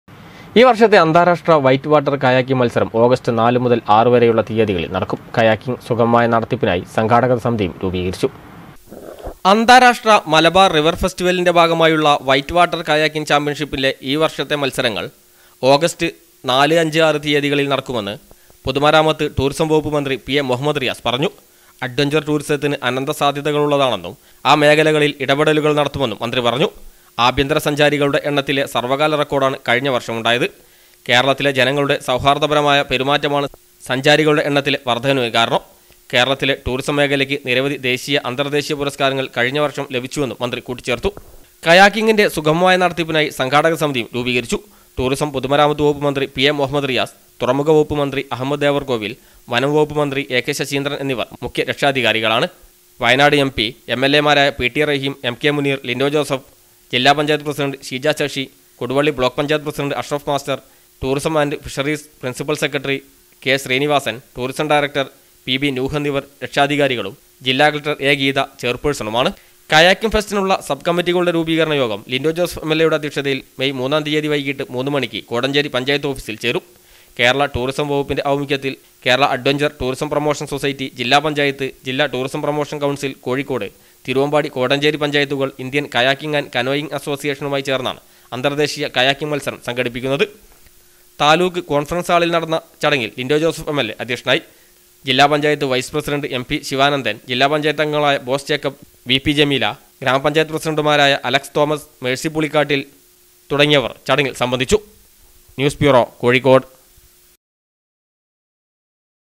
ഈ വർഷത്തെ അന്താരാഷ്ട്ര വൈറ്റ് വാട്ടർ കയാക്കിംഗ് മത്സരം ഓഗസ്റ്റ് നാല് മുതൽ 6 വരെയുള്ള തിയ്യതികളിൽ
ഈ വർഷത്തെ അന്താരാഷ്ട്ര വൈറ്റ് വാട്ടർ കയാക്കിംഗ് മത്സരം ഓഗസ്റ്റ് നാല് മുതൽ 6 വരെയുള്ള തിയ്യതികളിൽ നടക്കും.  കയാക്കിംഗിൻ്റെ സുഗമമായ നടത്തിപ്പിനായി സംഘാടക സമിതിയും രൂപീകരിച്ചു.